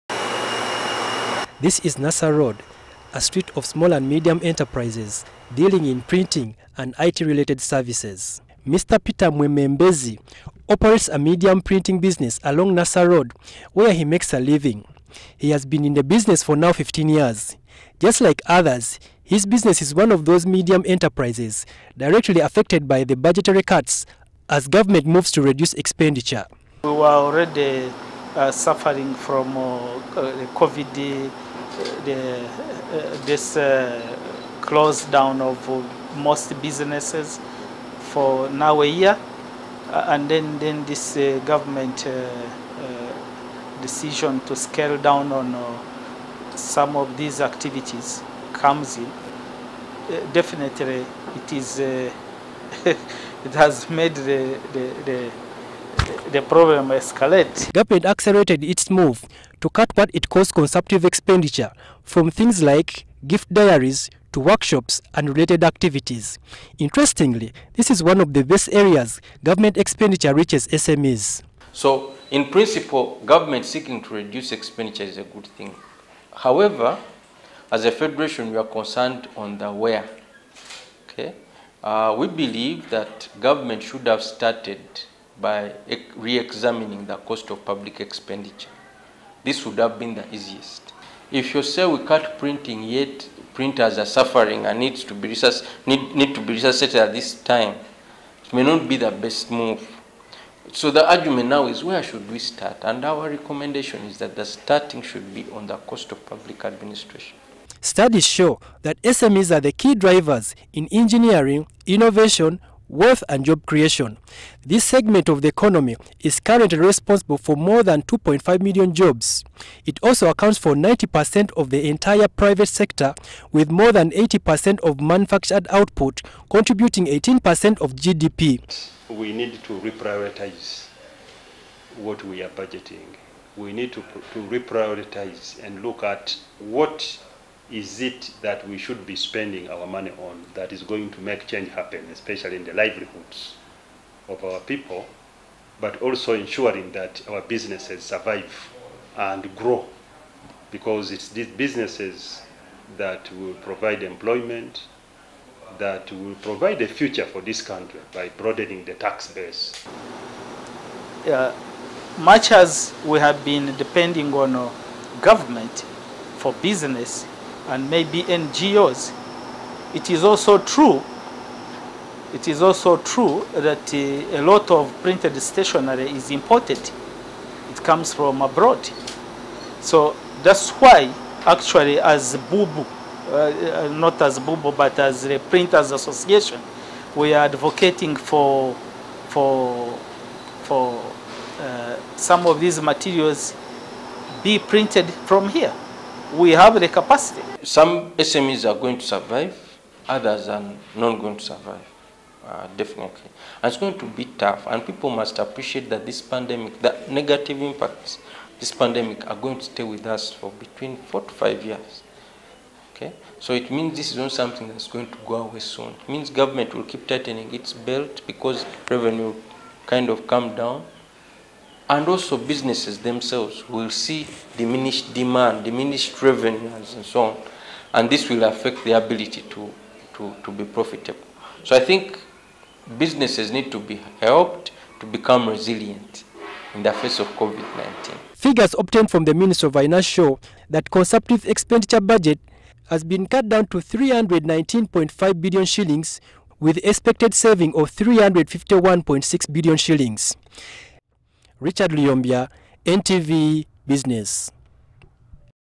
CONSUMPTIVE EXPENDITURE: Small and medium enterprises doubly impacted
When the government was rationalizing expenditure, workshops and gifts plus calendars were struck off the purchase lists. Unknowingly, these are the few areas by which SMEs tap into government expenditure. For SMEs, this is a double tragedy as Richard Luyombya reports